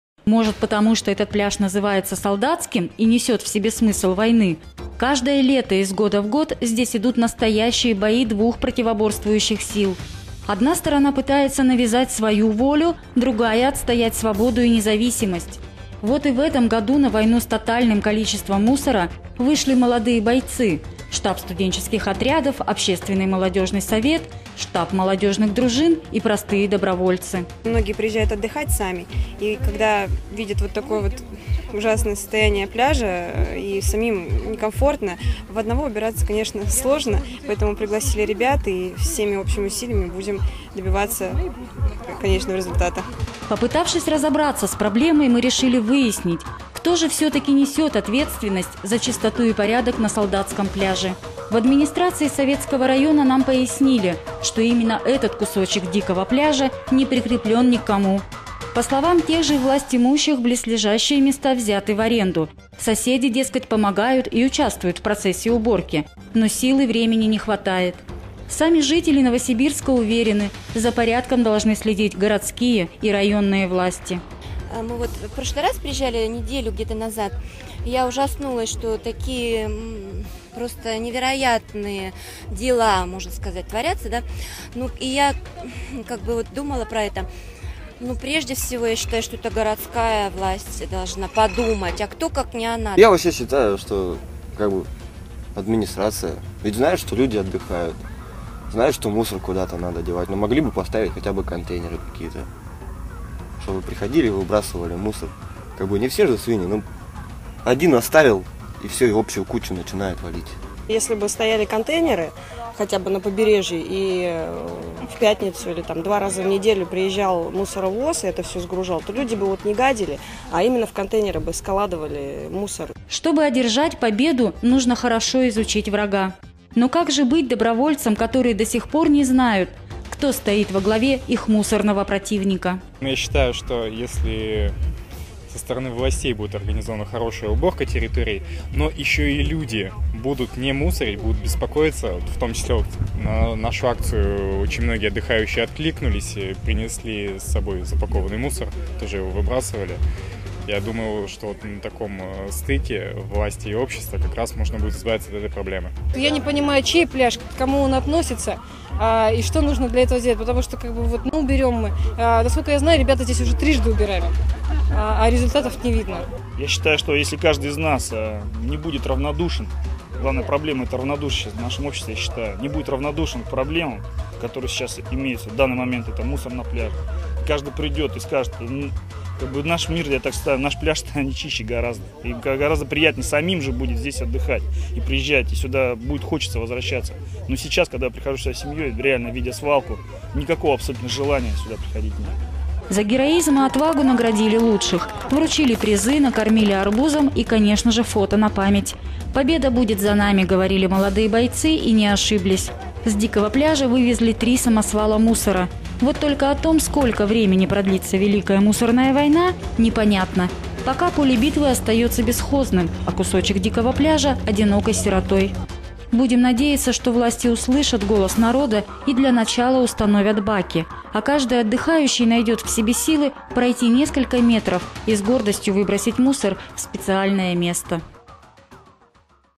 Война на Солдатском пляже.
На Солдатском пляже  Новосибирский Штаб Студенческих Отрядов совместно с ОМС и Городским штабом Добровольных Молодежных Дружин провели экологическую акцию «Чистый берег» при поддержке комитета по делам
молодежи мэрии города Новосибирска. Партнерами акции выступили: ООО "Кока-Кола ЭйчБиСи Евразия", председатель РОО «Наш Городок» Агафонов Виктор Леонидович и директор ГУП ЖКХ ННЦ СО РАН Распопов Сергей Николаевич.
Акция собрала порядка 60 добровольцев и совместными усилиями с пляжа было вывезено 3 полных самосвала мусора.